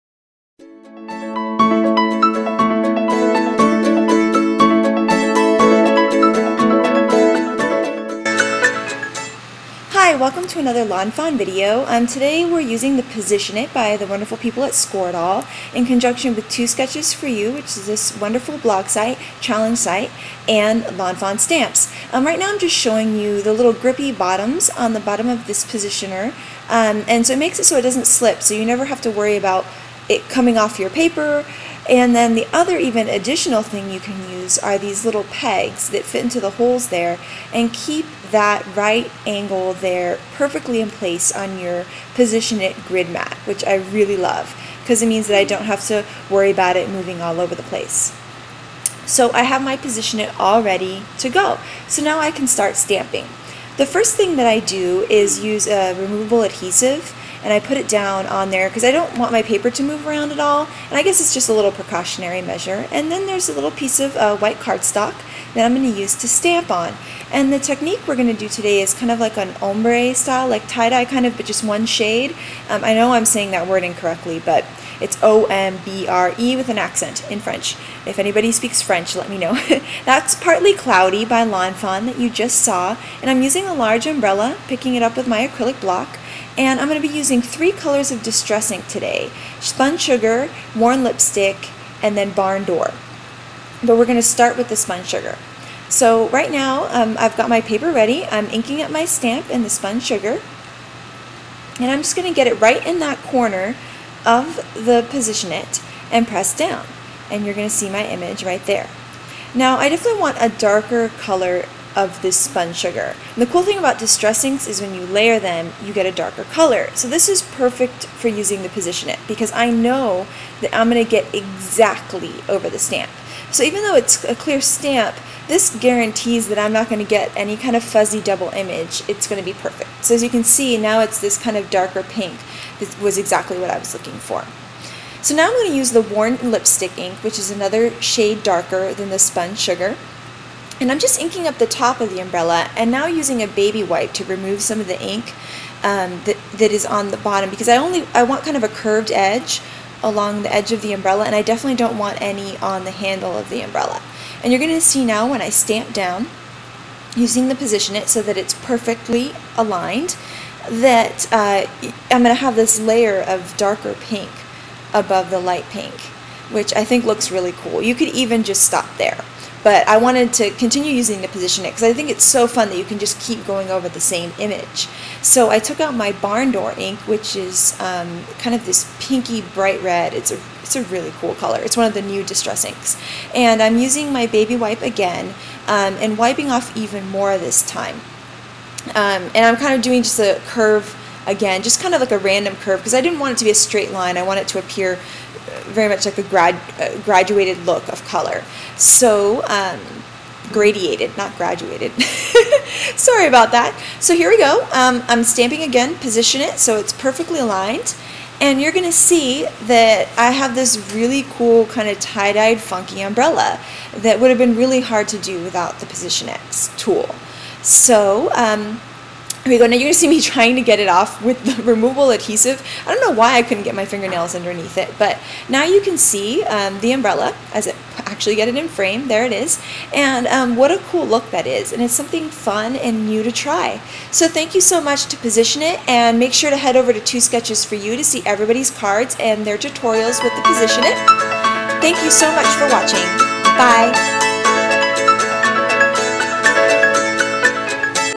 Lawn Fawn: Using the Position-It to make a tie-dyed / ombre stamped look
In this video I use the Position-It tool and Lawn Fawn's Partly Cloudy stamp set in conjunction with 2 sketches 4 you to make a tie-dyed / ombre look on a stamped umbrella image.